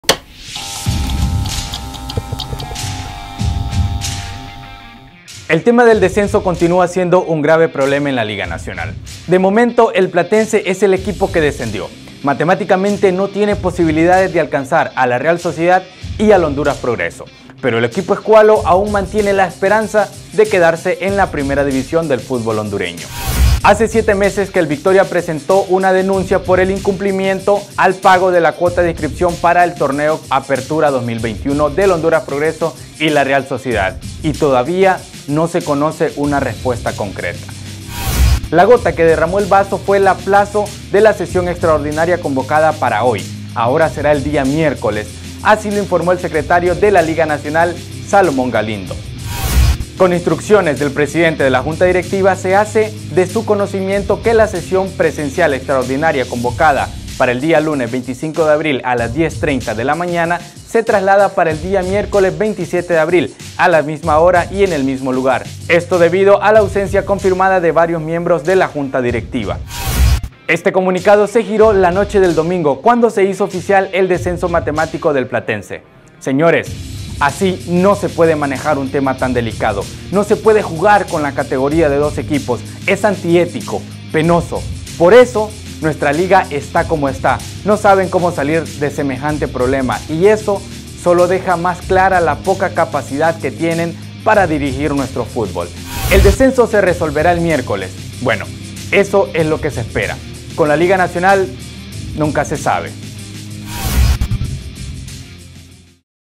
No saben cómo resolver el tema del descenso 👎🏼 LigaNacional Honduras Fenafuth Descenso #2022
La Liga Nacional dejó pasar mucho tiempo el tema del descenso y ahora parece que no sabe cómo resolverlo, a falta de una fecha para que termine el Clausura todavía se desconoce quién será el descendido: Platense o la Real Sociedad.